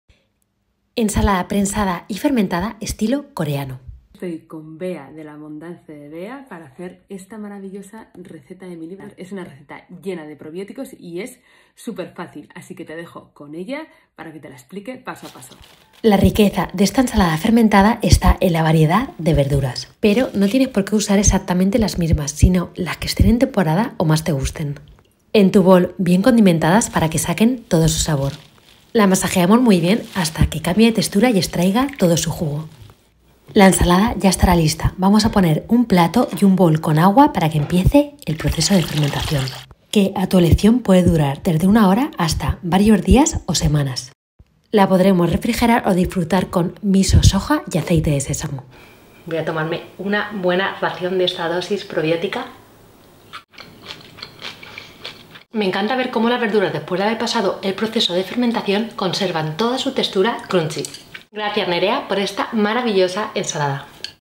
Ensalada coreana, prensada de col fermentada con Nerea Zorokiain
Os traemos esta ensalada fácil y variada, rica en probióticos y de sabores.
Es ideal, pues puedes variar ingredientes según temporada, lo que tengas por casa o lo que más te guste. Por ejemplo añade col normal en vez de china, partida muy finita. Incorpora rábano, hinojo, etc.

Ingredienes:
- 200 g de col china
- 100 g de col lombarda
- 20 g de cebolla roja
-  150 g de zanahoria
-  1 cebolleta
-  50-60 g de pepino pelado sin pepitas
-  1 diente de ajo
-  1 cucharadita de sal marina
-  1 cucharadita de azúcar de coco
-   2 cucharaditas de vinagre de arroz
- 2 cucharaditas de gochugaru o pimentón picante o normal
Para decorar:
- cilantro fresco. También puedes usar albahaca.
- 2 cucharadas de semillas de sésamo tostado
Aliño (todo bien emulsionado):
- 1 o 2 cucharaditas de tamari o salsa de soja
-   2 cucharaditas de miso blanco
-  2 cucharaditas de aceite de sésamo tostado
Elaboración:
1. Corta en láminas muy finas las coles y la cebolla roja puedes usar una mandolina-, y en juliana la zanahoria, la cebolleta y el pepino.
2. Échalo todo en un bol y anade el ajo picado muy fino, la sal, el azúcar, el vinagre y el gochugaru.
3. Mezcla bien todos los Ingredientes con las manos, apretando ligeramente, hasta que las hortalizas empiecen a soltar líquido.
4. Pon el contenido en una prensa para vegetales o coloca un plato encima con peso. Enseguida verás que el líquido cubre las hortalizas troceadas.
5. En una hora ya estarán listas, pero las puedes dejar que fermente varios días, incluso semanas. Siempre en un lugar fresco y oscuro. Puedes tapar el bol con un paño. Siempre bien cubierto con su jugo.
6. Una vez fermentada puedes refrigerar o consumir.
7. En el momento de consumir, agrega el aliño de miso, aceite de sésamo y  tamari. Mezcla muy bien. Sirve con las semillas de sesamo tostadas y el cilantro fresco por encima.
Ensalada súper adictiva.

 lAbondancedebea 🤍